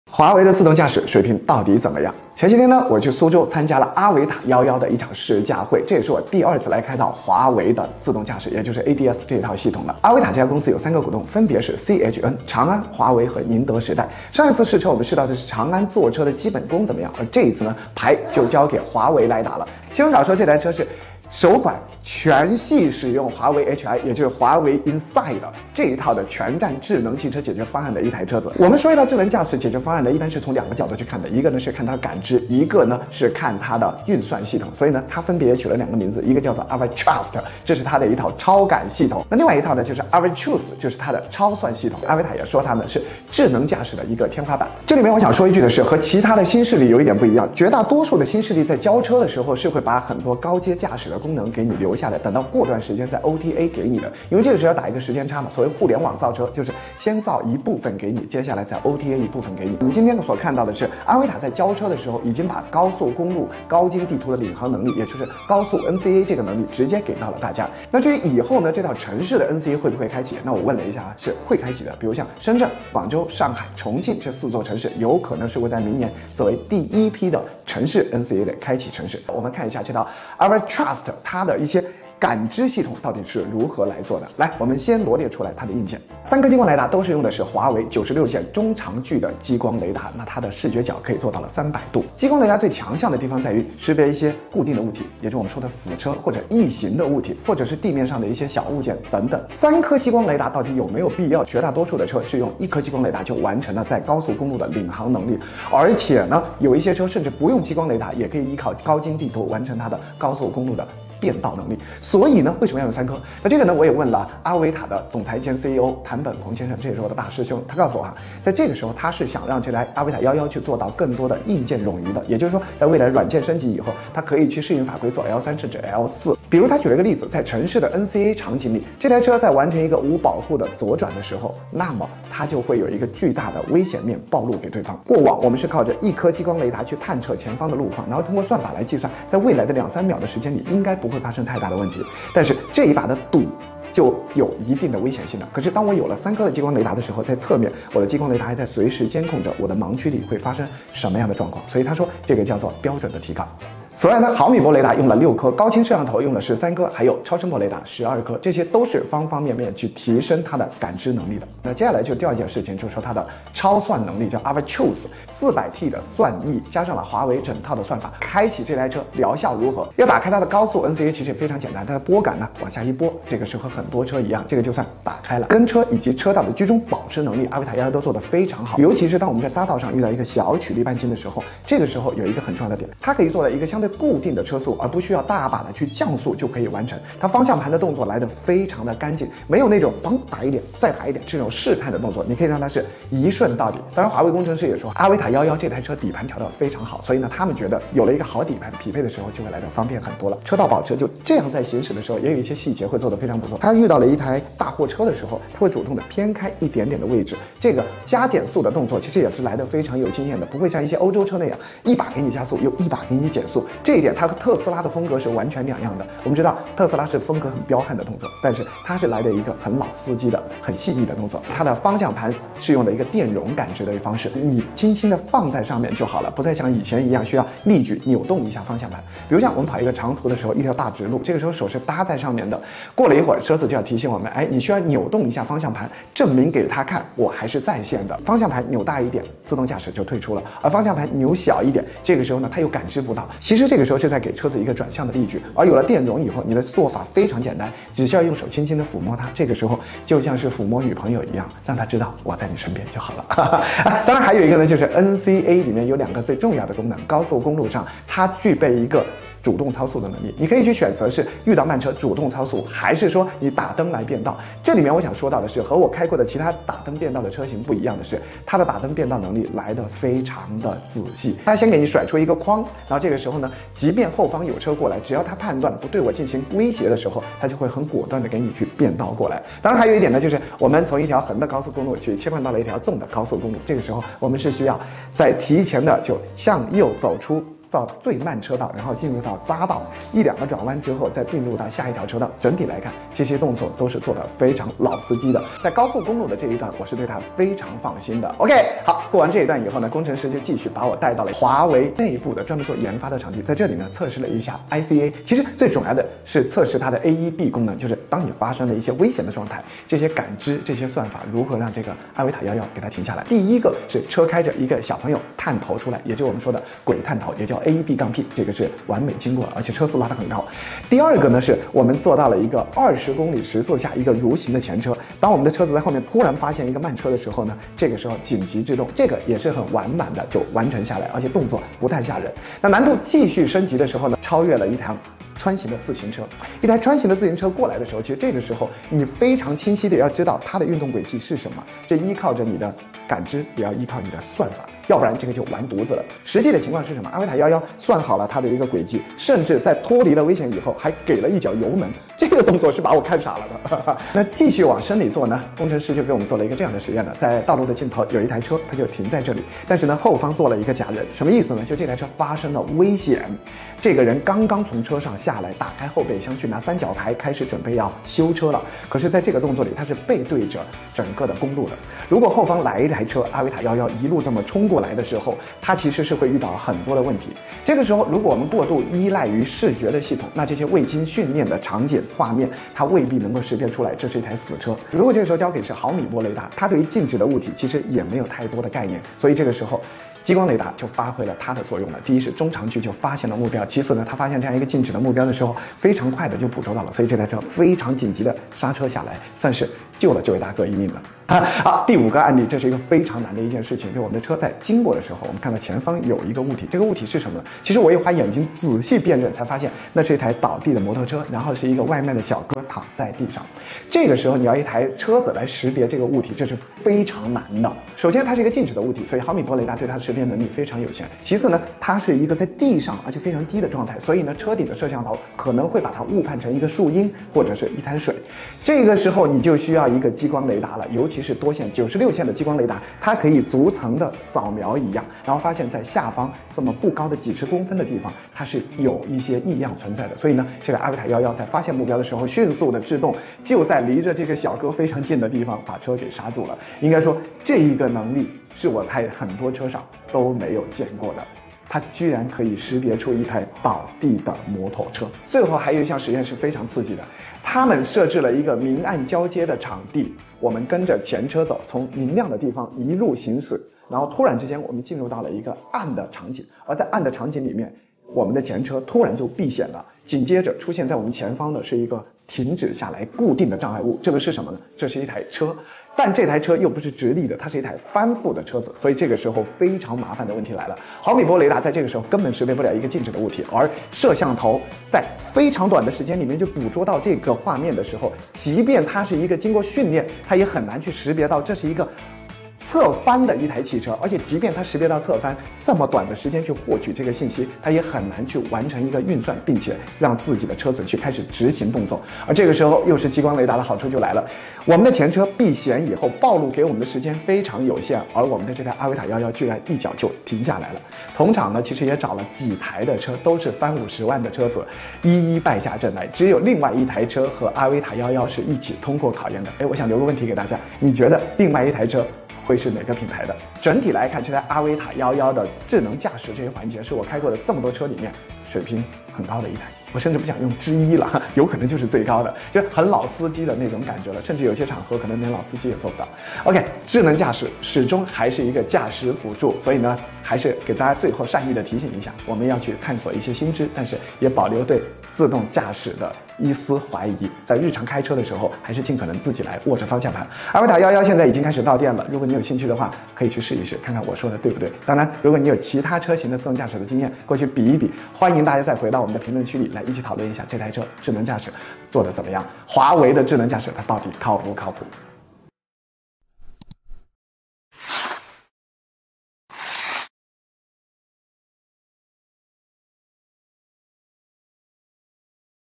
华为的自动驾驶技术到底怎么样？
_________________★独家授权★_________________
阿维塔AVATR有三个股东：长安+华为+宁德时代，HUAWEI Inside全家桶里就包括自动驾驶，光是激光雷达就给了三颗。那作为唯一一家进入华为试验场内部进行多项测试的媒体，我就要认真帮你们试试了！